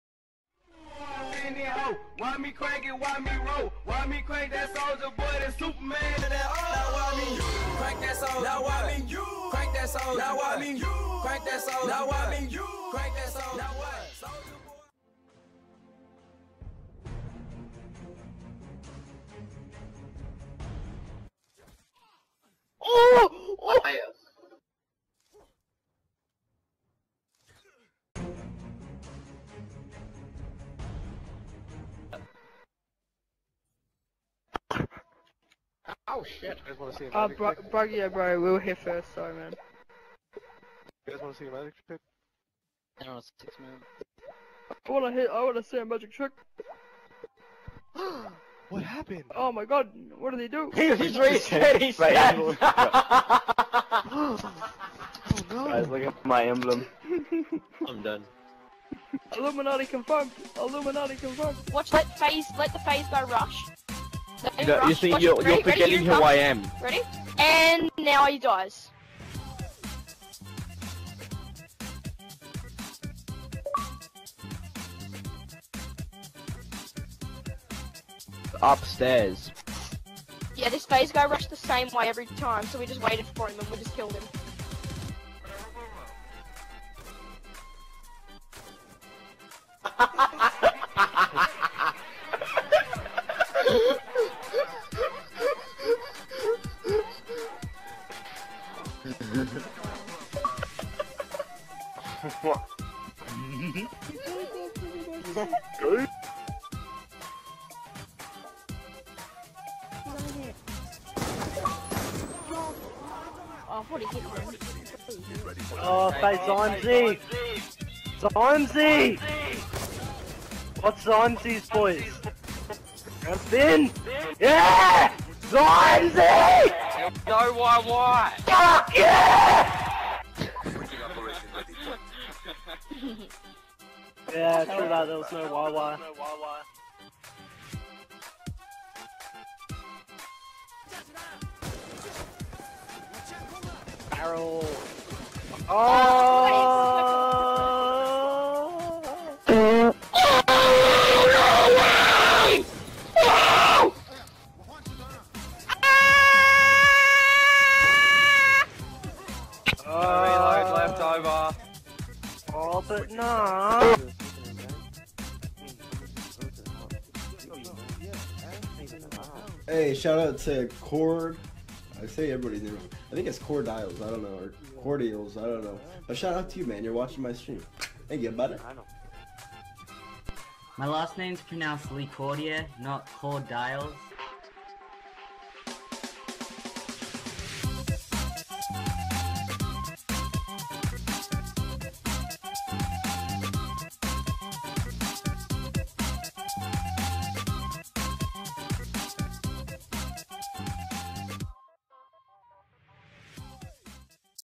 Funnies And Fails #2
This is Funnies And Fails #2!!!

Due to all the support that episode one got of 'Funnies And Fails' I found a whole bunch of funny clips from the past and decided to edit all the clips together and make something outta nothing and create Funnies And Fails #2. Hope you enjoyed :)

Add me on Snapchat: masonlecordier